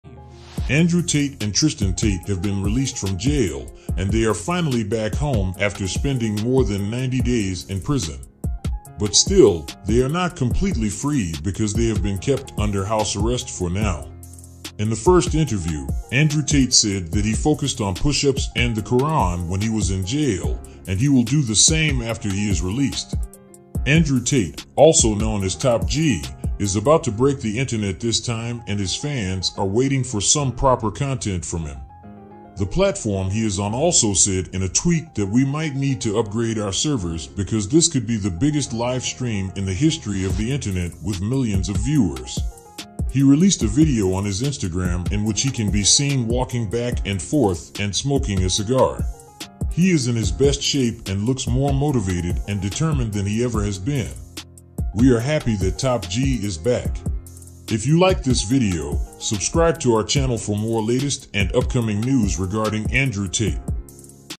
Andrew Tate is Back with a BANG (New Update) 🔥
Andrew Tate and Tristian Tate also known as Tate Brothers are released from jail after 95 days. Both are controversial internet personalities charged of human trafficking's case. They are not found guilty and are released from jail on house arrest. Many of their fans came for their support.
Email for credit or removal.